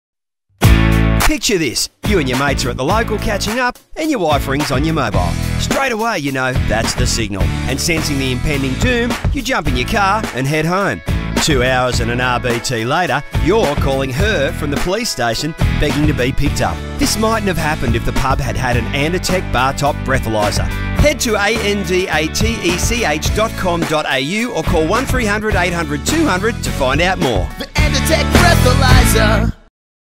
Drinks at the Bar - Andatech AlcoSense Breathalyser Ad
Picture This: You and your mates are at the local catching up, and your wife rings on your mobile.

Straight away you know that's the signal, and sensing the impending doom you jump in your car and head home. Two hours and an RBT later, you're calling her from the police station begging to be picked up.

This mightn't have happened if the pub had had an Andatech Bar Top Breathalyser!

Andatech Breathalyser